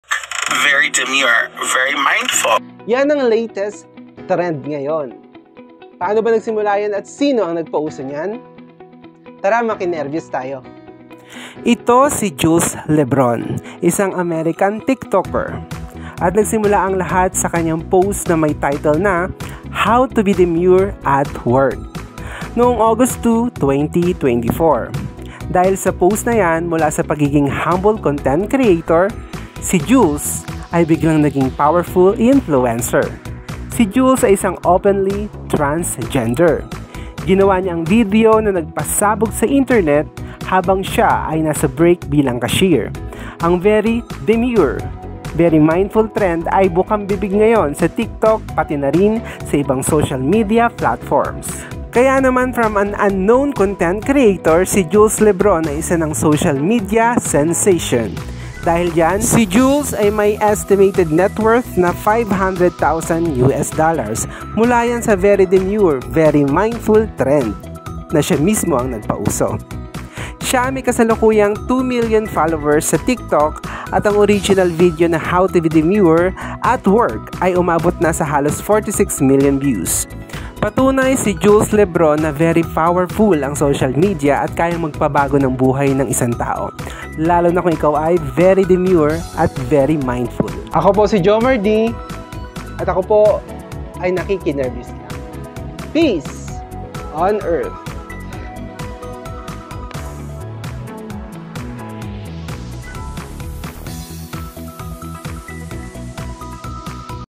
Very Demure,Very Mindful TREND
Alamin kung paano at sino ang nagsimula ng trend na'to.